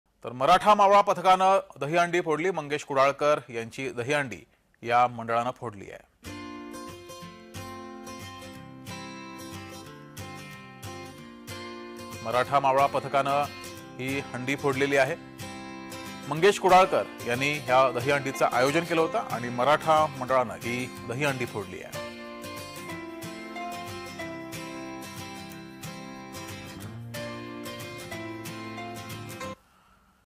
Mangesh Kudalkar | मराठा मावळा पथकानं फोडली दहिहंडी | Marathi News
मराठा मावळा पथकानं फोडली दहिहंडी